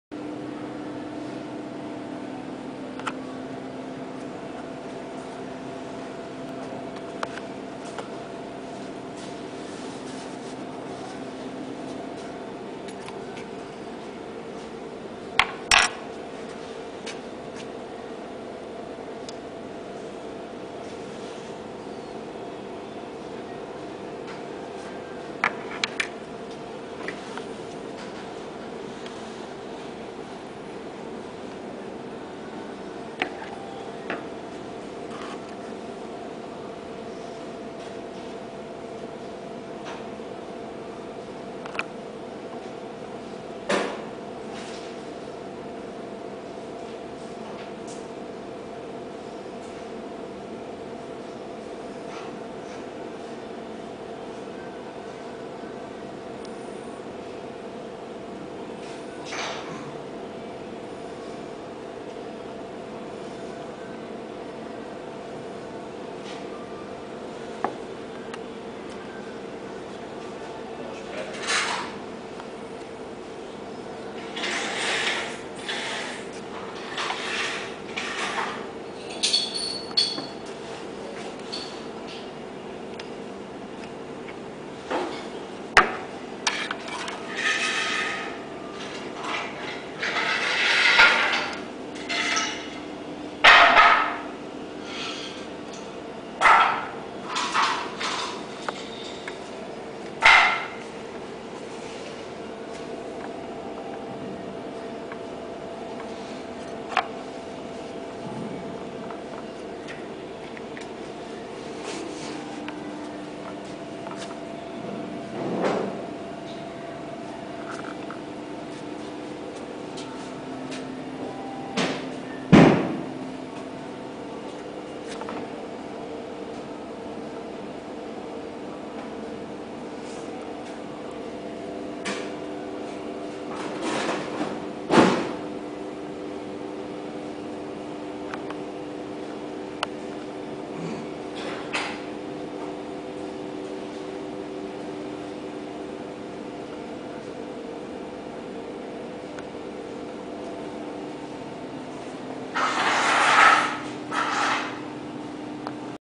AMETEK Brookfield viscosity test for Bitumen application
Standard Test Method for Viscosity Determination of Asphalt at Elevated Temperatures Using a Rotational Viscometer